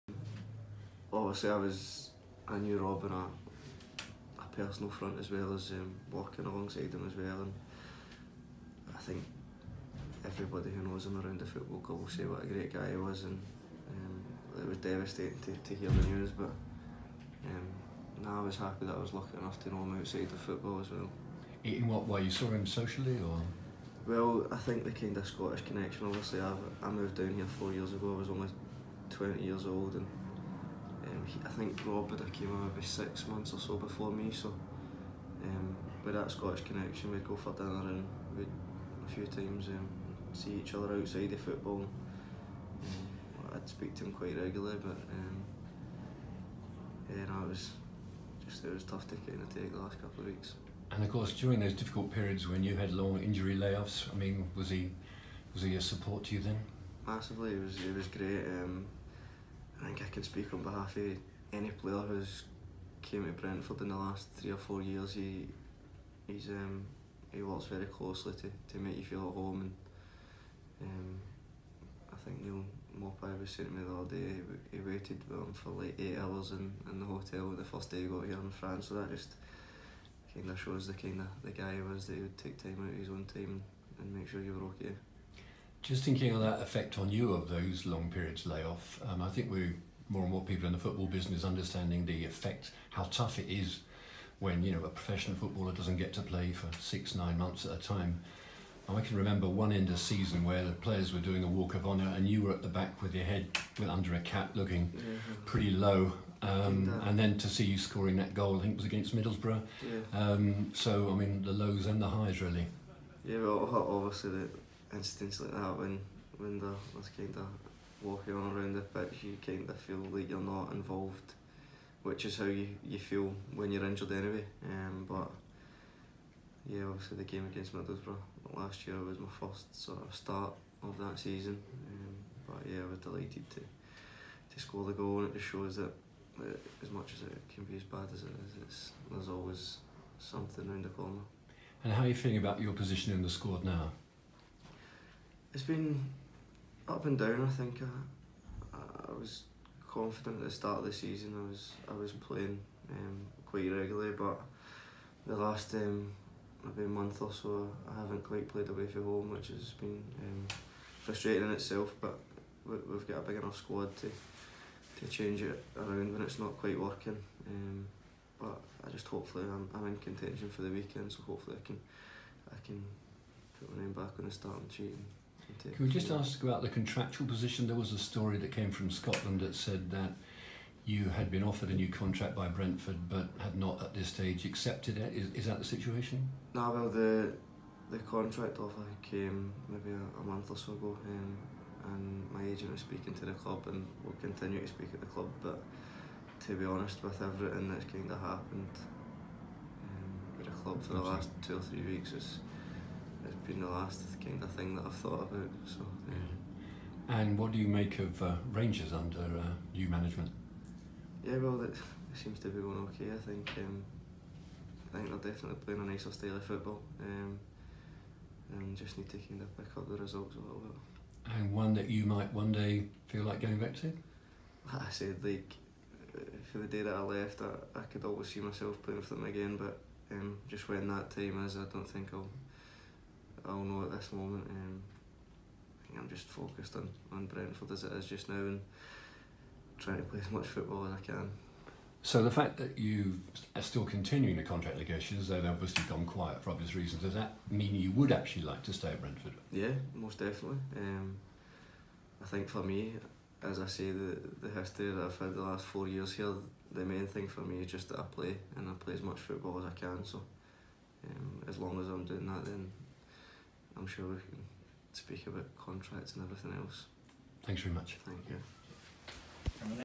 Lewis Macleod
Brentford midfielder talks about the late Rob Rowan and his own career at Brentford